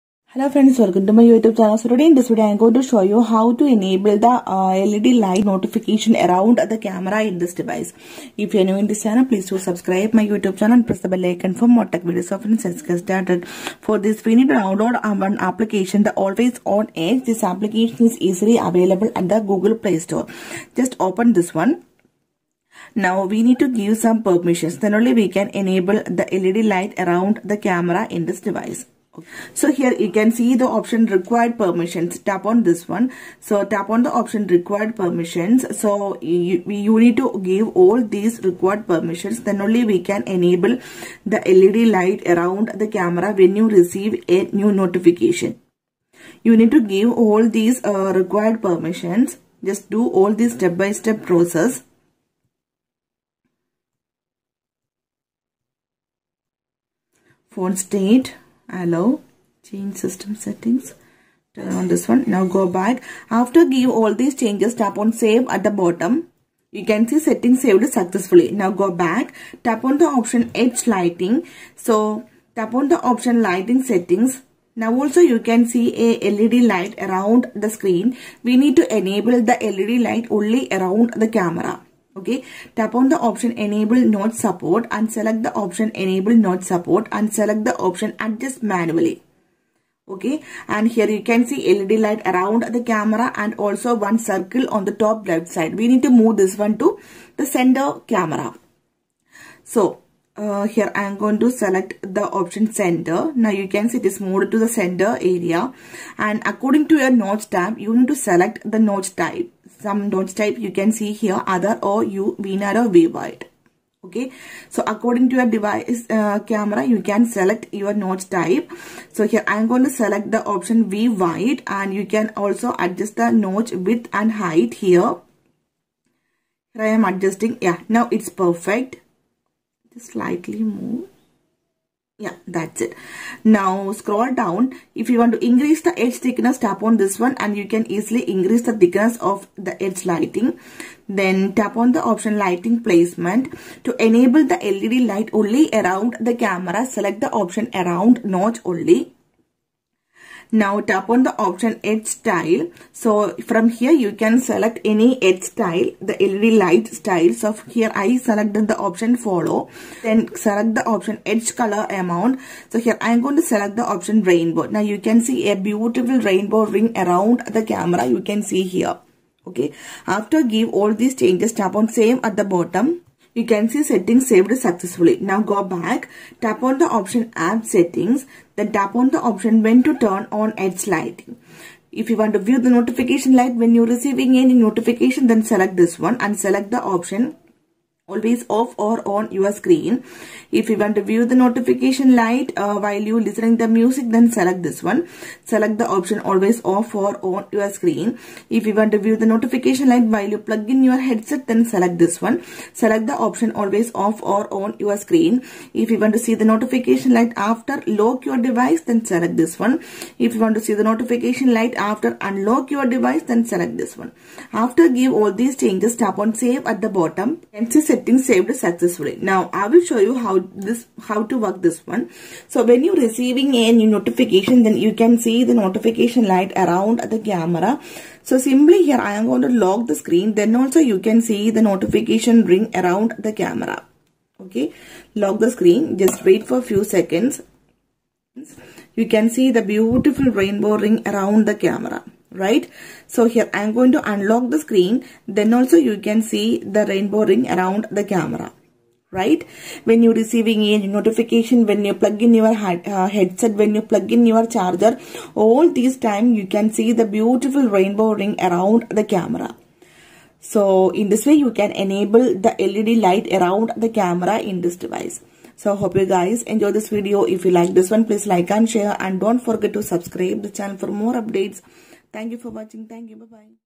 Tecno Spark 8T | How To Enable LED Light around Camera in Tecno Spark 8T
Hello friends this video shows you that " How to enable LED light around camera in Tecno Spark 8T "